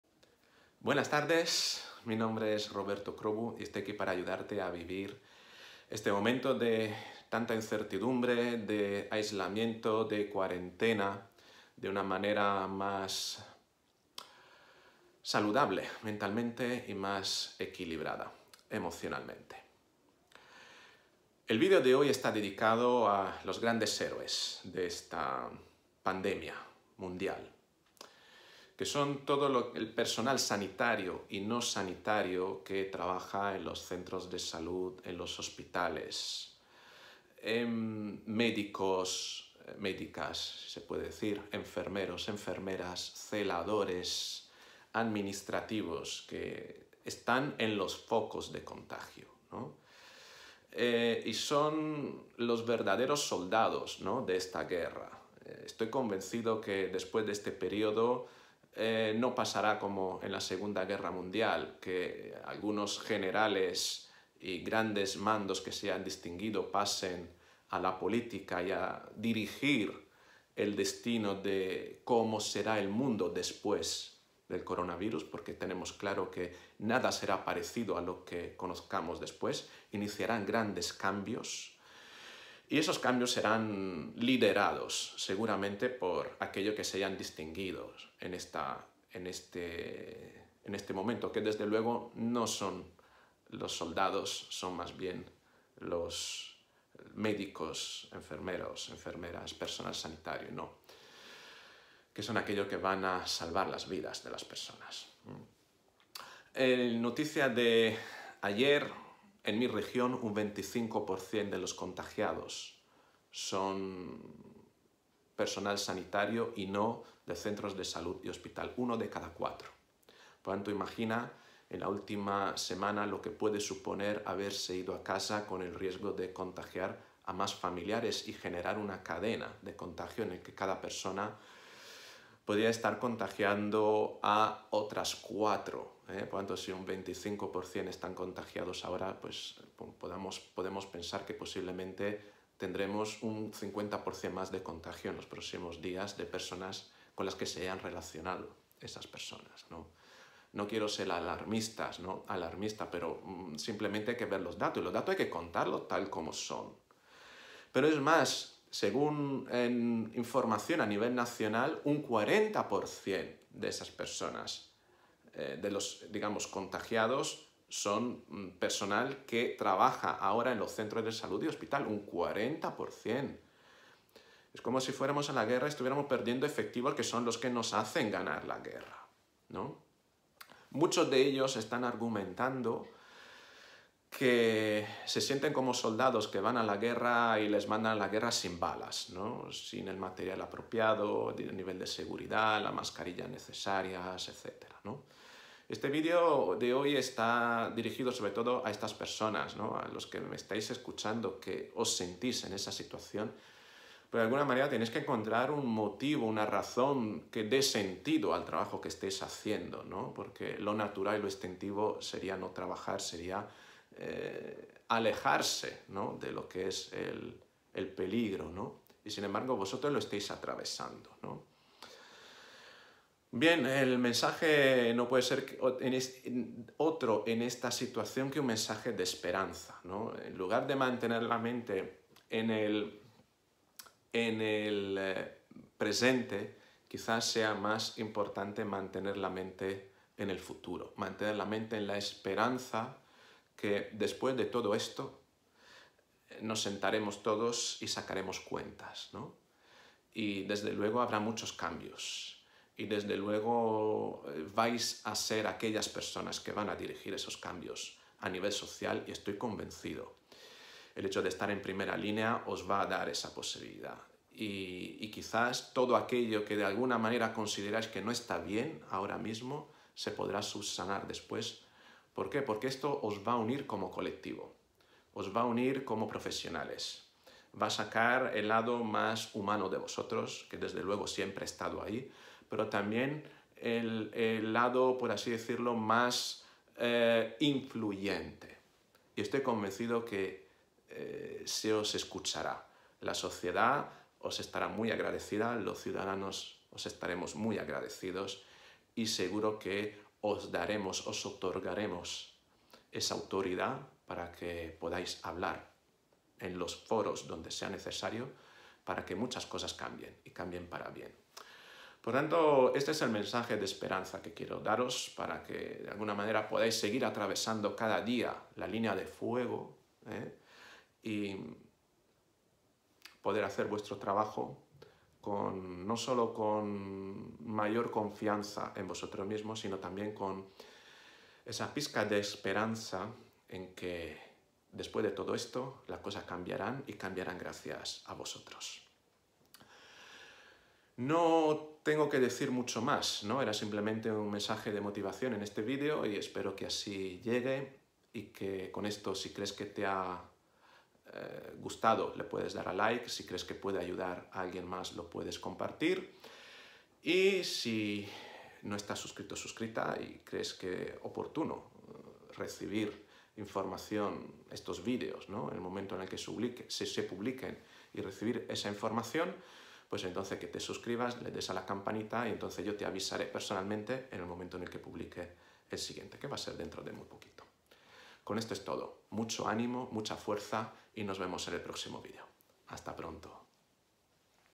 Cómo Motivarte si Eres Sanitari@ - Un Mensaje de Esperanza para Nuestros Héroes
Tal vez sean de poco consuelo mis palabras. Tal vez no motiven. Pero creo que en situaciones como las que están viviendo nuestros profesionales de la sanidad y salud en primera línea de batalla, incluyendo el tan olvidado personal auxiliar no sanitario, es necesario, ahora más que nunca, encontrar un sentido existencial: un para qué, por el que valga la pena vivir este momento.
Ellos son nuestros héroes. Lo mejor que podamos hacer para facilitarles el trabajo es quedarnos en casa. Nos podrán ayudar si les dejamos que nos ayuden. Mucho ánimo y fuerza. La sociedad española no olvidará esto. Y apoyará los cambios que impulsaréis en este país cuando todo esto acabe. Estoy convencido de ello.
Mi nombre es Roberto Crobu, soy Psicólogo.